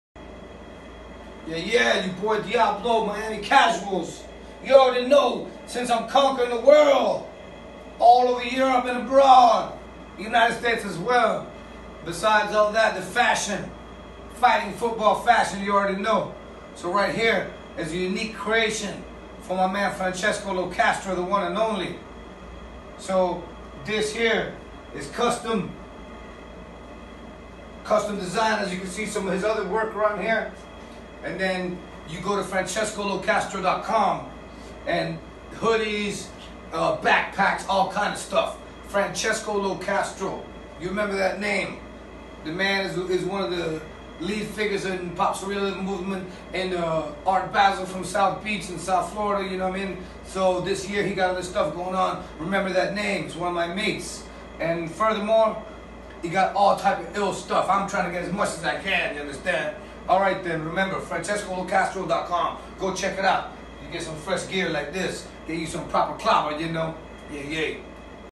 Art of the World ep 1- Francesco Locastro
International Artist FRANCESCO LOCASTRO is a multi-platform Artist, herein showing off a piece on the sculpted frame of one DEREK DIABLO aka DEL BOY D- MIAMI CASUALS.